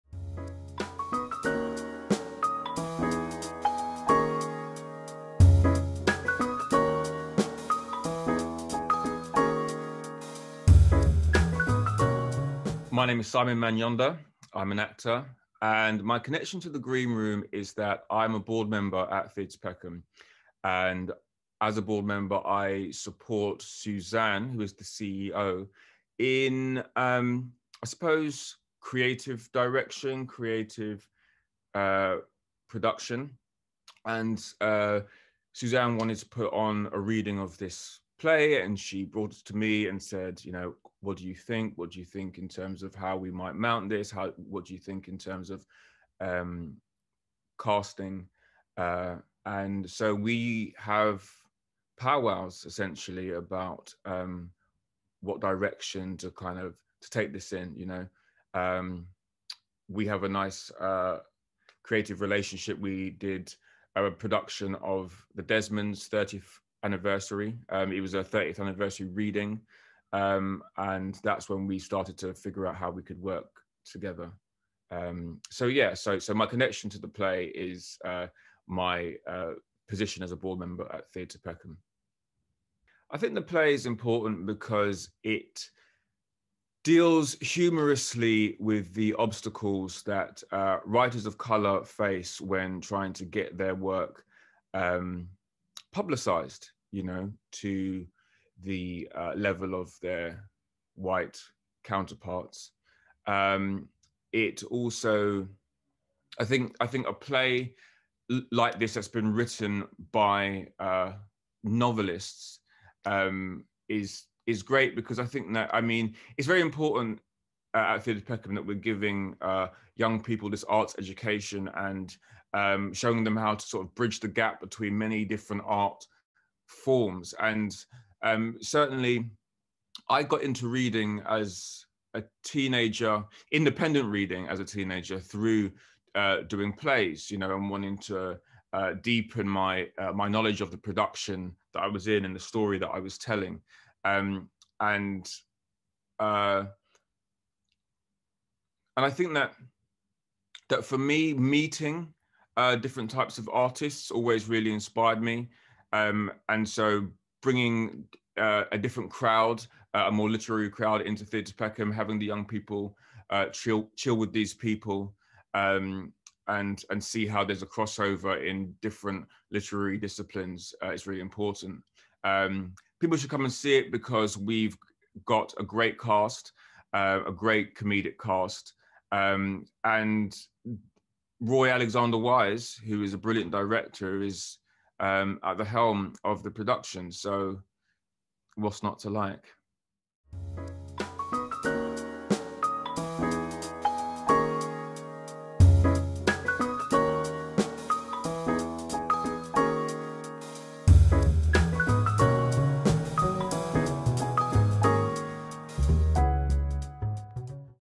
The Green Room - Simon Manyonda
Award-winning authors Alex Wheatle, Yvvette Edwards and Irenosen Okojie explore the publishing industry’s ‘diversity’ record by centring the narratives of writers of colour in The Green Room, an original play to be live streamed from Theatre Peckham (Sunday 4th July 2021).

The Green Room is a comedy-drama about six authors, at different stages of their careers, who have been nominated for a diversity in literature award. As they wait in the green room for the awards ceremony to start, the authors’ egos, secrets and disappointments are gradually revealed.

A laugh-out-loud romp, directed by award-winning Theatre Director Roy Alexander Weise (Nine Night), The Green Room is a timely and clear-eyed exploration of the publishing industry, and the obstacles new and established writers of colour in Britain face while trying to carve out a career.